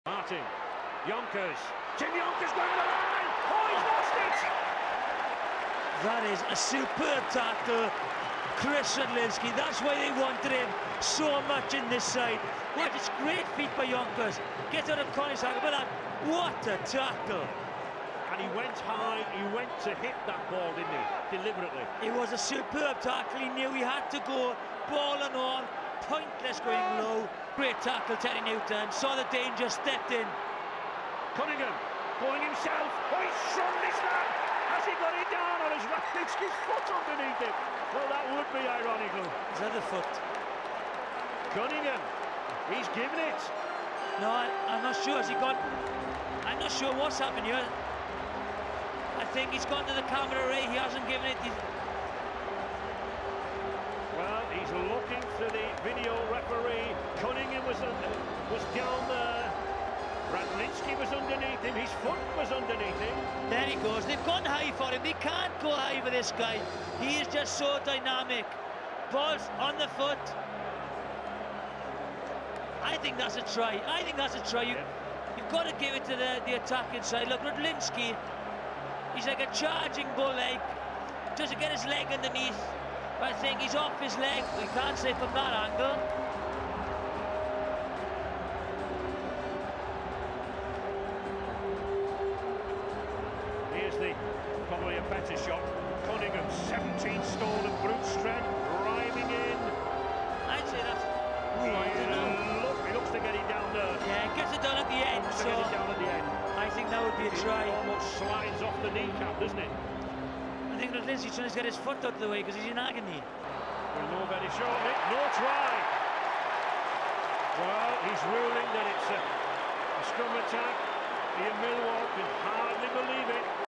Kris Radlinski - The Foot of God
If Maradona had the Hand of God, Kris Radlinski had the Foot of God in the 2002 Challenge Cup Final. Despite having been hospitalised with an infected foot just before this match he played to win the Lance Todd Trophy in Wigan's 21-12 success over St Helens.